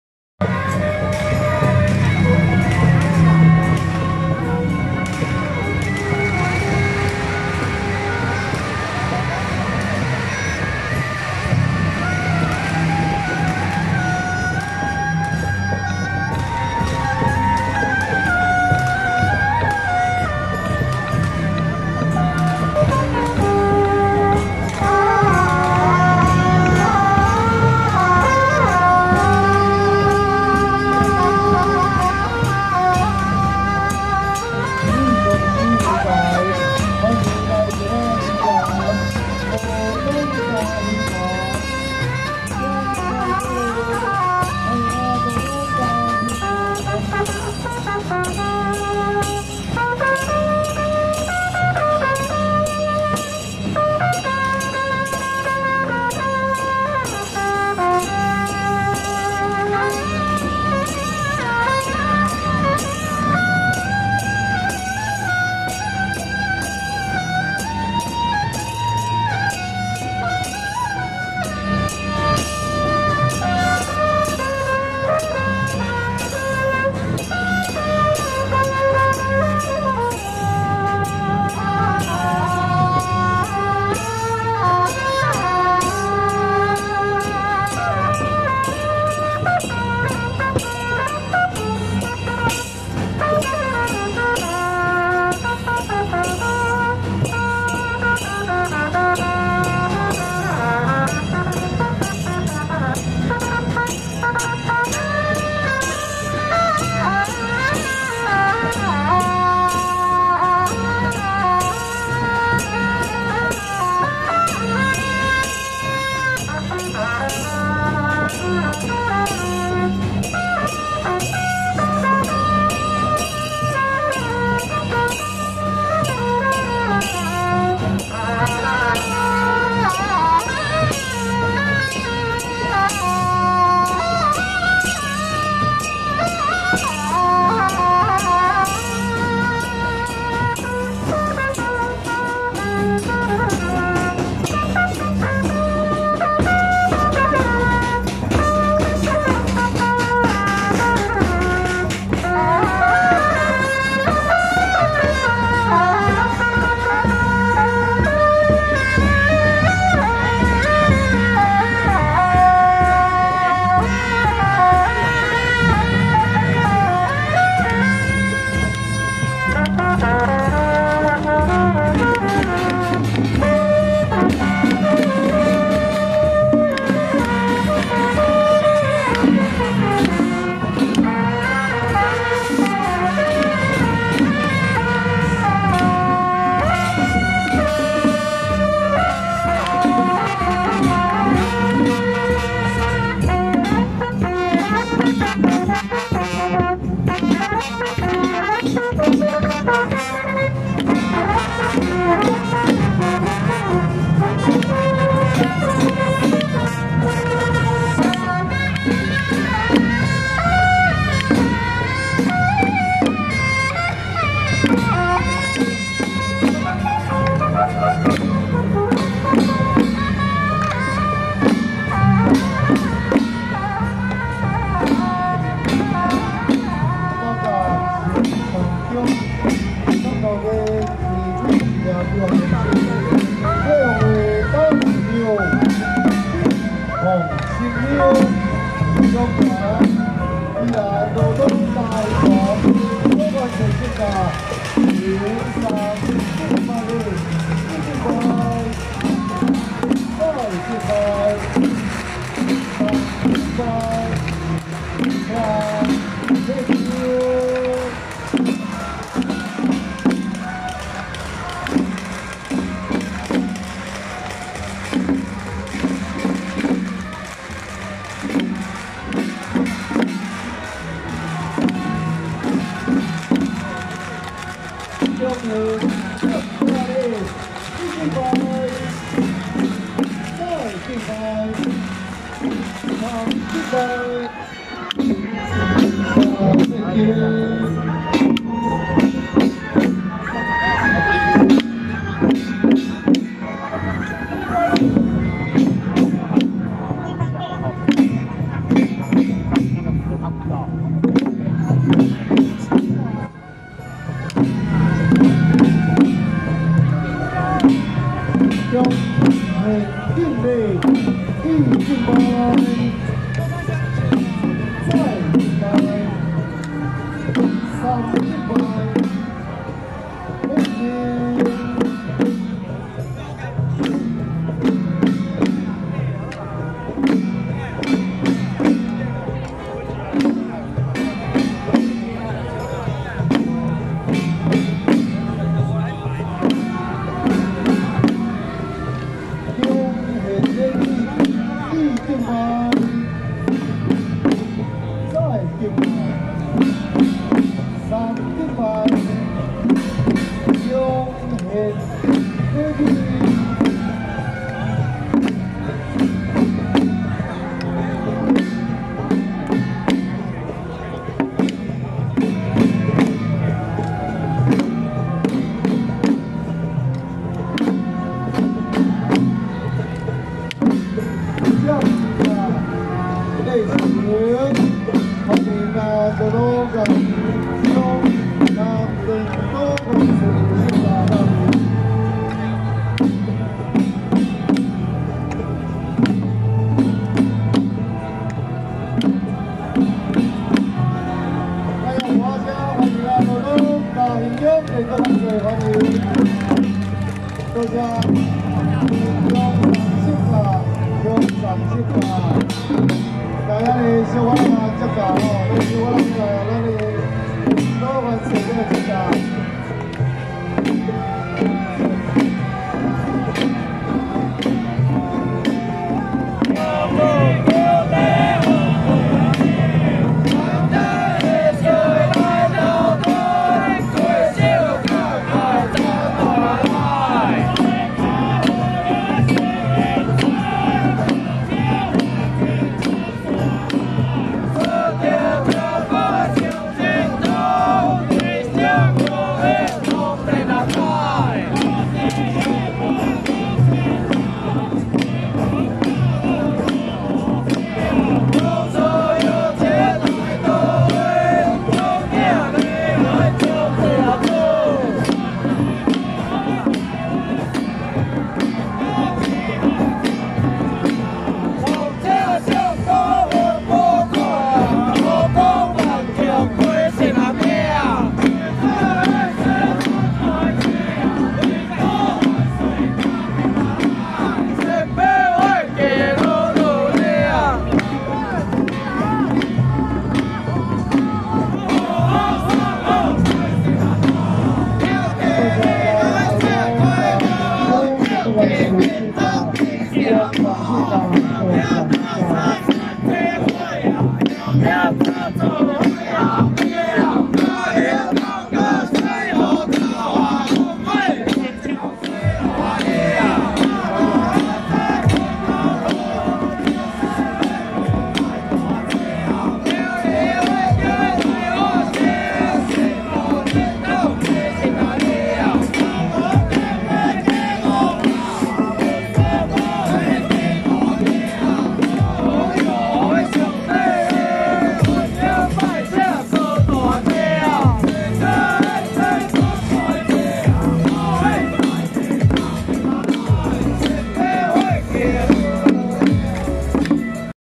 【羅東嘉興團-鮮花車中西樂＆田都元帥 聖駕(員山廣興堂+臺北双后宮+汐止詹家關聖帝君+台北協敬堂轎班團)】~109艋舺青山宮恭祝靈安尊王聖誕暨建宮165週年正日遶境
羅東嘉興團-鮮花車中西樂＆田都元帥 聖駕(員山廣興堂+臺北双后宮+汐止詹家關聖帝君+台北協敬堂轎班團)